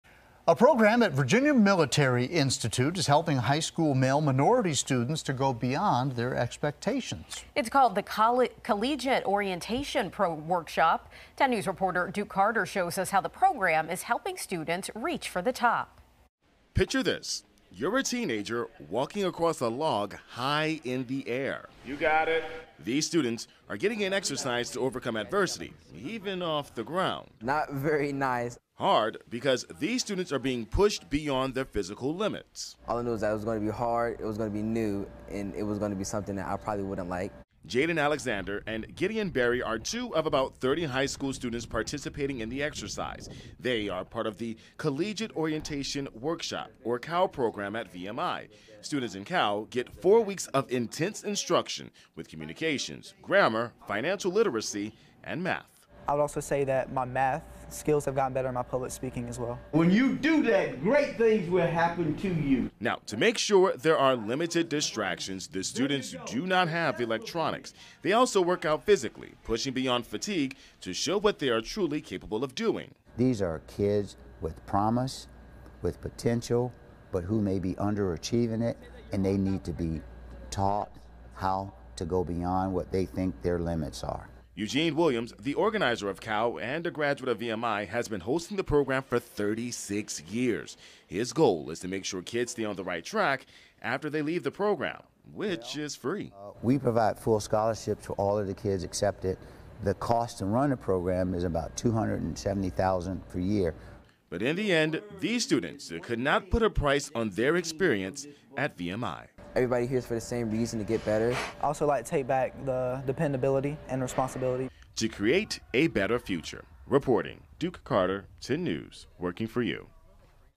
Collegiate orientation workshop held at VMI
Students are getting pushed out of their comfort zones to get prepared to their futures.